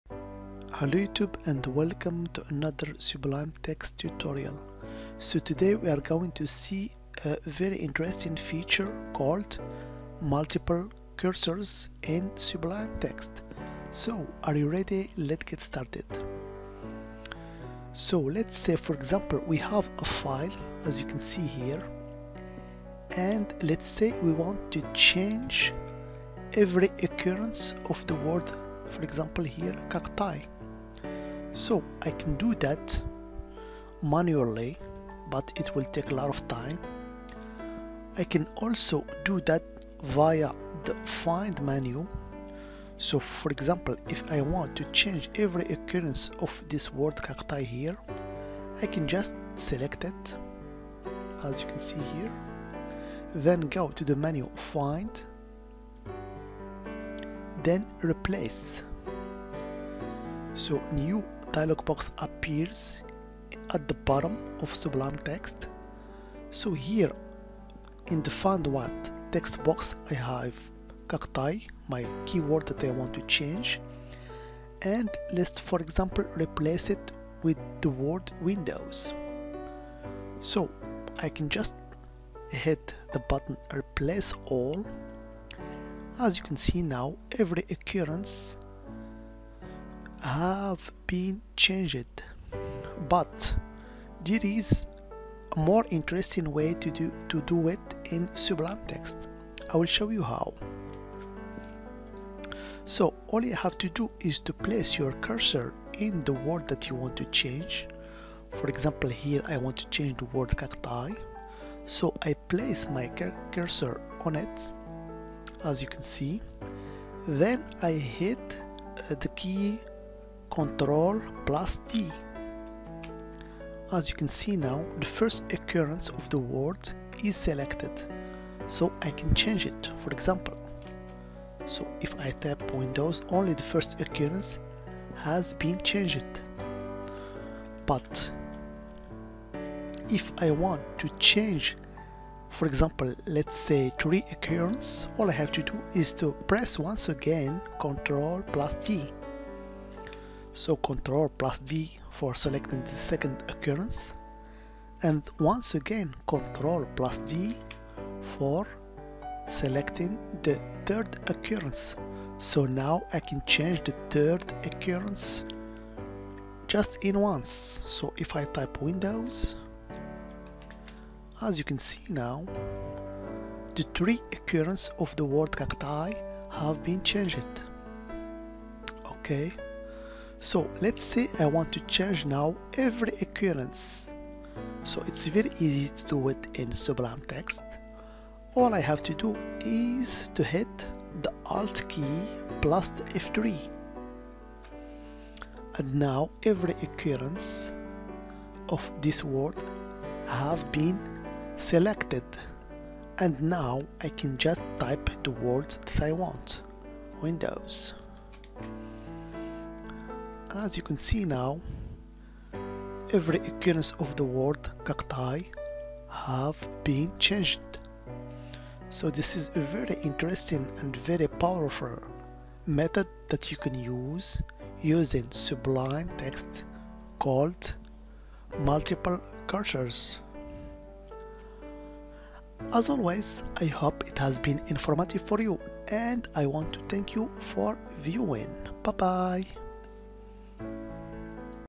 Sublime Text 3 : How to use Multi-Cursors on Windows
Multiple cursors are a killer feature of Sublime Text. It allows you to type in multiple places at the same time, saving you development time.
To enable multiple cursors, put your cursor on the word you want highlighted and hit CTRL+D till you have selected all the words you want.
You can also create multiple cursors with ALT+F3 to highlight all occurrences of the word you want to change.